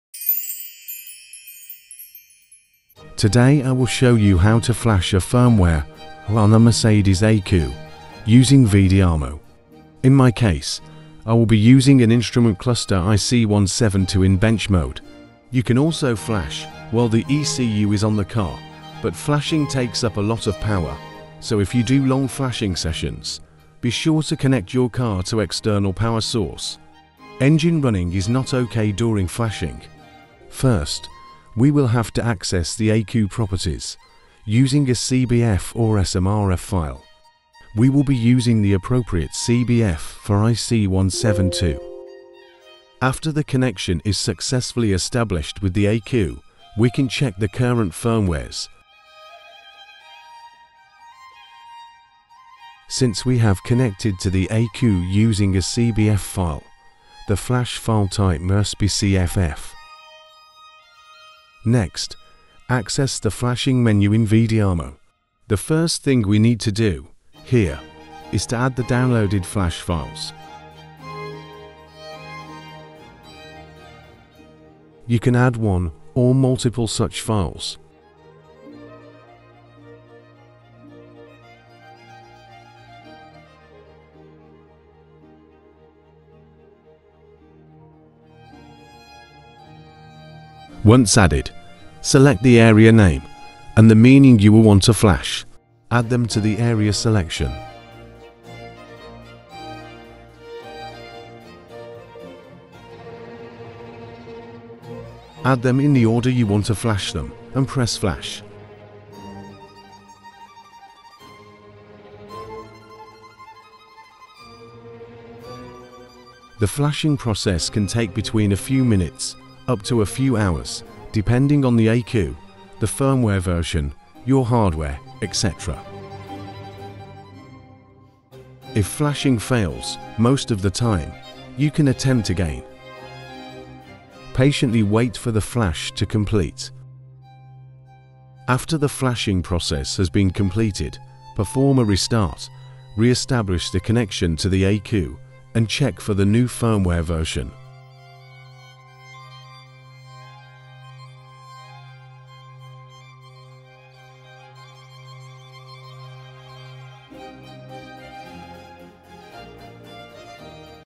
How to Mercedes ECU flashing via Vediamo
This video describes the detailed steps of how to use Vediamo engineer software to program the Mercedes-Benz 172 instrument